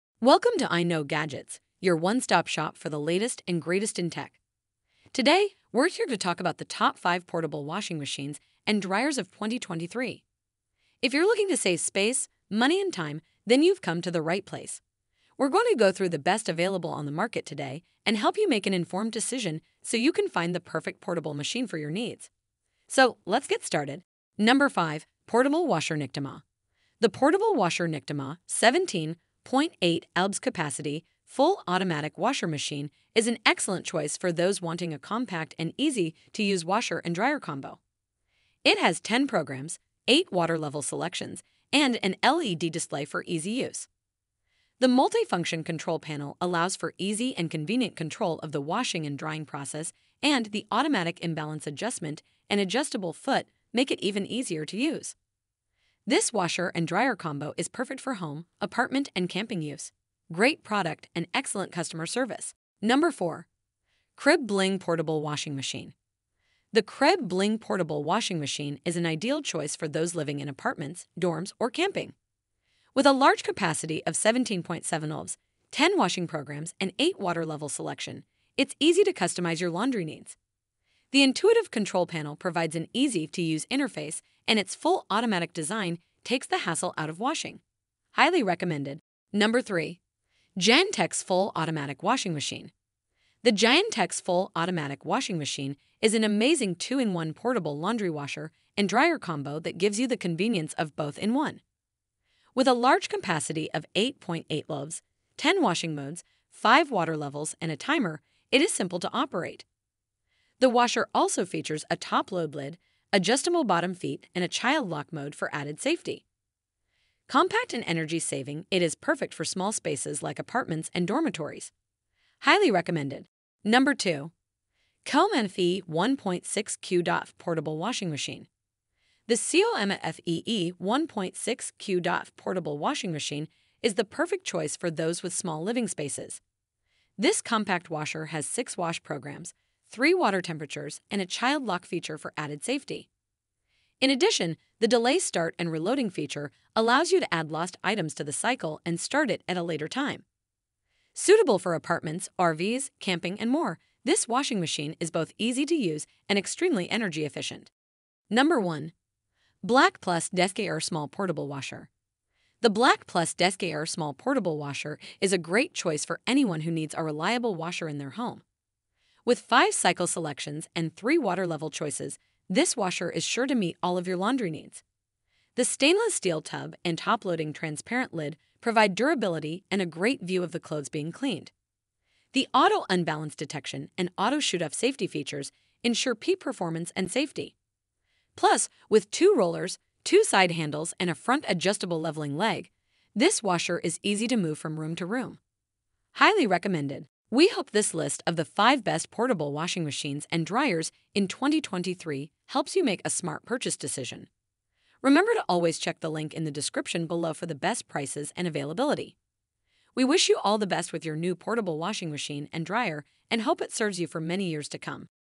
TOP 5 Best Portable Washing Machines and Dryers of 2023
➜ Discover the Best 5 Portable Washing Machines and Dryers of 2023 on Amazon! At Valid Consumer, we've researched and curated a list of top-notch Portable Washing Machines and Dryers of 2023, saving you time and money. Click the links below to learn more and make your purchase.

0:00 Intro

00:25 ➜ 5. Portable Washer Nictemaw
●Amazon US
01:03 ➜ 4. Krib Bling Portable Washing Machine
●Amazon US
01:30 ➜ 3. Giantex Full Automatic Washing Machine
●Amazon US
02:04 ➜ 2. COMFEE' 1.6 Cu.ft Portable Washing Machine
●Amazon US
02:38 ➜ 1. BLACK + DECKER Portable Mini Washing Machine
●Amazon US

click through and make a purchase, I may receive a
commissions support content creation like this. Thank you